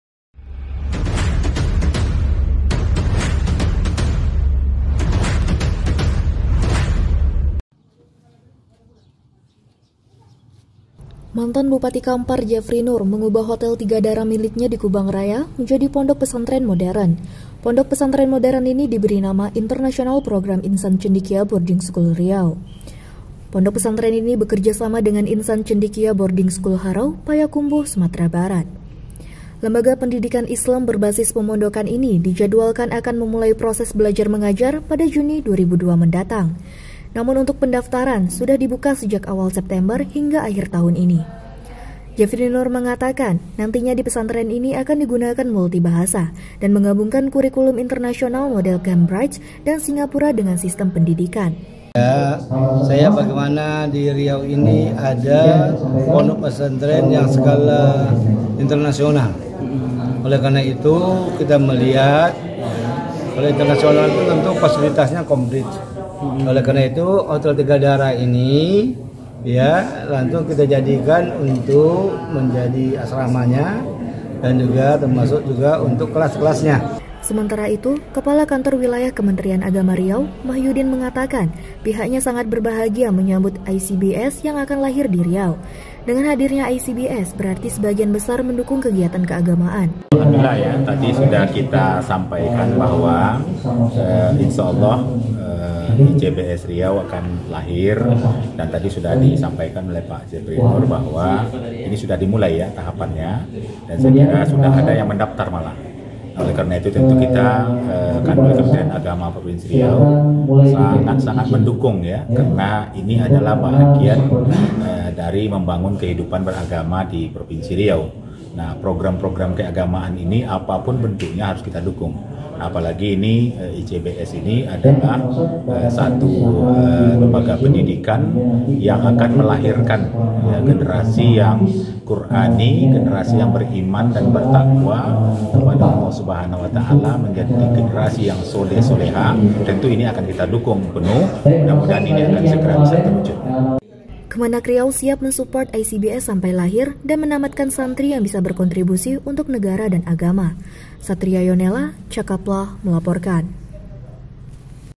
MANTAN BUPATI KAMPAR UBAH HOTEL TIGA DARA MENJADI PESANTREN ICBS RIAU
PEKANBARU (CAKAPLAH) - Mantan Bupati Kampar Jefry Noer mengubah Hotel Tiga Dara miliknya di Kubang Raya menjadi Pondok Pesantren modern, yang diberi nama International Program (IP) Insan Cendikia Boarding School (ICBS) Riau.

Pondok pesantren ini bekerjasama dengan Insan Cendikia Boarding School (ICBS) Harau, Payakumbuh, Sumatera Barat.

Lembaga pendidikan Islam berbasis pemondokan ini dijadwalkan akan memulai proses belajar-mengajar efektifnya pada Juni 2022 mendatang. Namun pendaftaran sudah dibuka sejak awal September hingga akhir tahun ini.